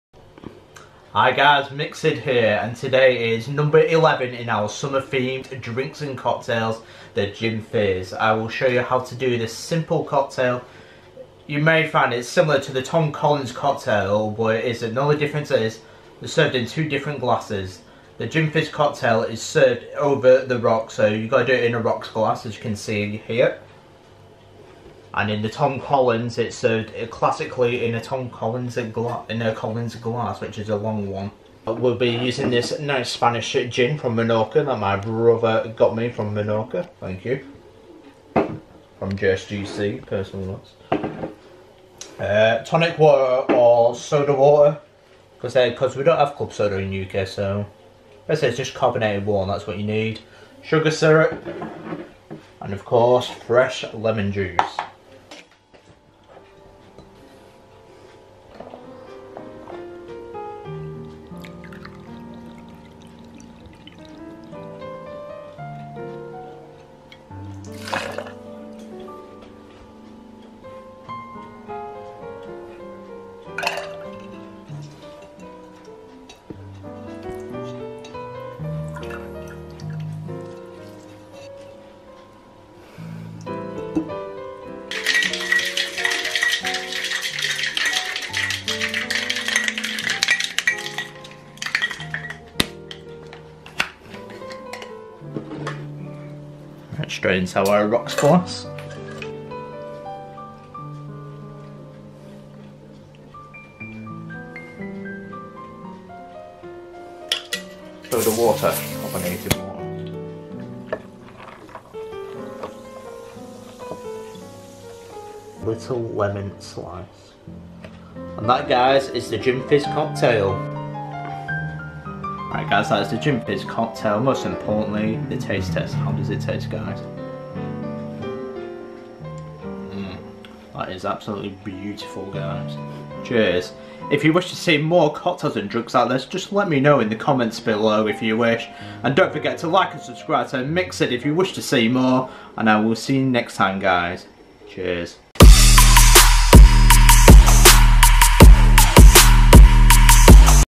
Summer Drinks #11 | How To Make The Gin Fizz
Number 11 in our summer themed drinks and cocktails the Gin Fizz. It is similar to Tom Collins with the recipe but the difference is the glassed they are served with the Gin Fizz served over the rocks whilst Tom Collins is served in a Collins glass.

Recipe:
50ml Gin
25ml sugar/simple syrup
15ml lemon juice
Top up with soda water - for example spring water or carbonated water which tonic water falls under.

Preparation:
1. Get a rocks glass or an Old fashioned glass and fill with ice.
2. Let's get started pour in 50ml of gin into the shaker.
3. Pour in 25ml of sugar syrup/simple syrup.
4. Next pour in 15ml of lemon juice
5. Shake vigorously for a good 10 seconds, then pour into the rocks glass.
6. Last ingredient is to add is the tonic water or soda water if you can find one to top up with.
7. Any good cocktail should have a garnish so decorate with a lemon slice.

That is the Gin FIzz cocktail number 11 in our summer themed cocktail and drinks. Let me know in the comments belowor my social media if there is any cocktails or drinks you want to see as I love trying new ideas and experiments for cocktails.